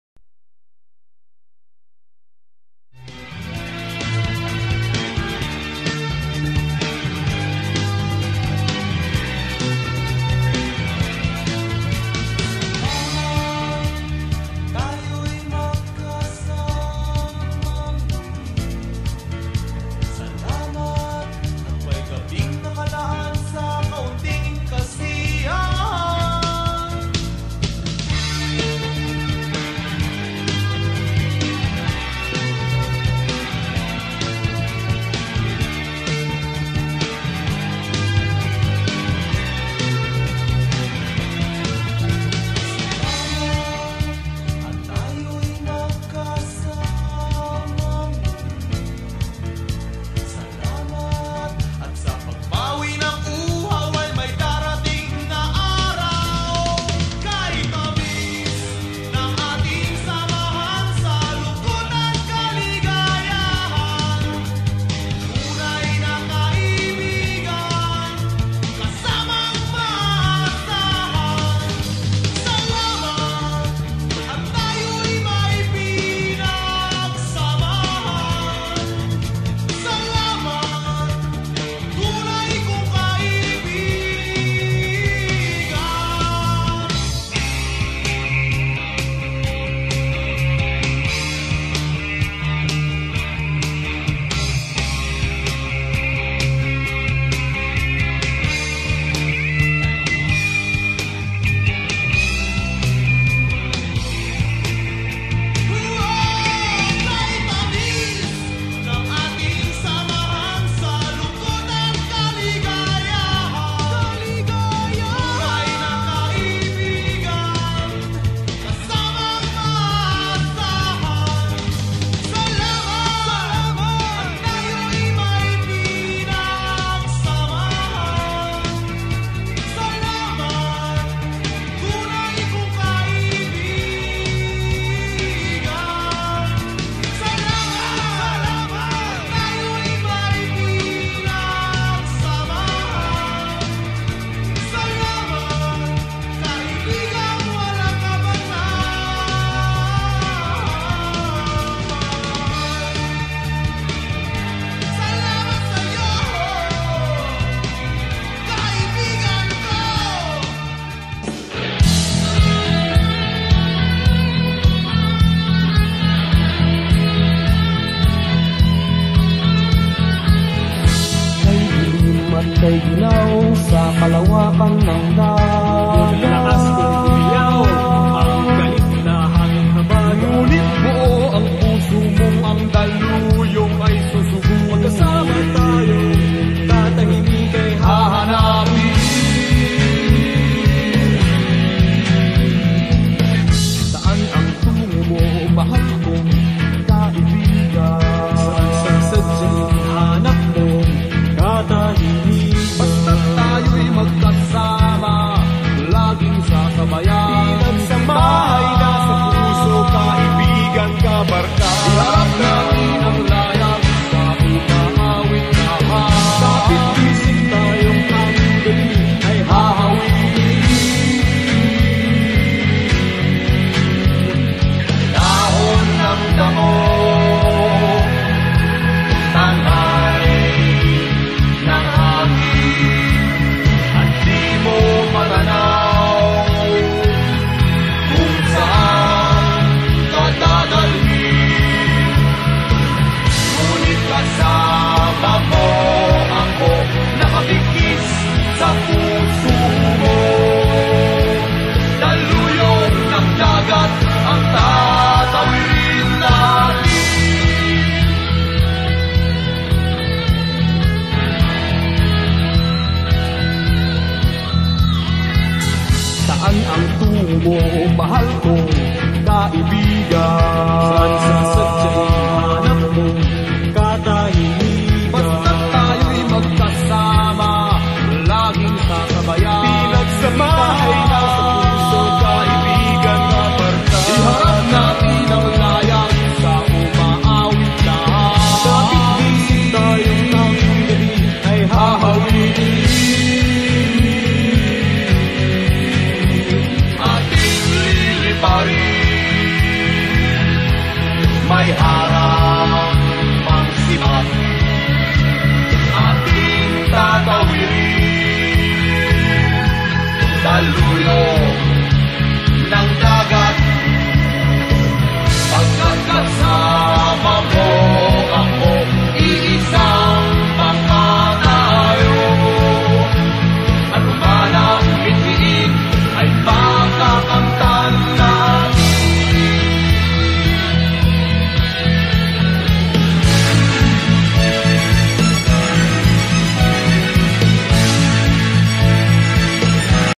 upsilon lambda phi
UPSILON LAMBDA PHI 1970 frat/sorro-  mix pic. ..samahang walang kapantay!!
Upsilon Lambda Phi founding anniversary held in Apha Beta Chapter Feb. 14, 2008